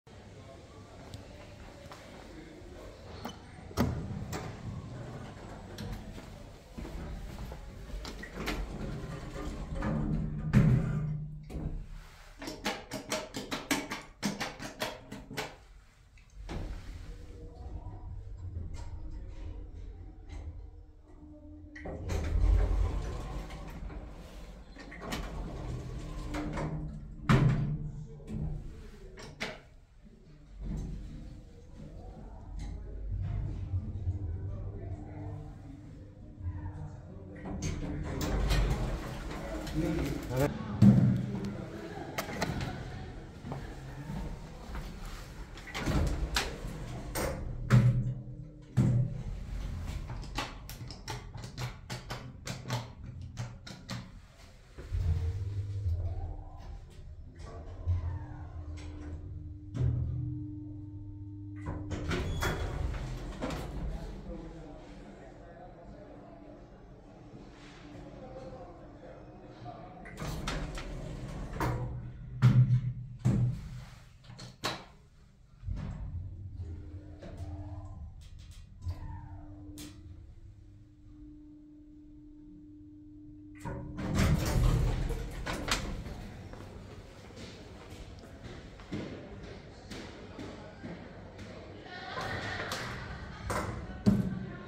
【2台並び】激レアエレメイト！だが…東芝製の古いエレベーター
かなり貴重な後期エレメイト。
未更新の交流二段制御ですが、2階止まりとなっています。
かつて上の階はホテルだったようで、ホテルが廃業し今では入れないようになっています。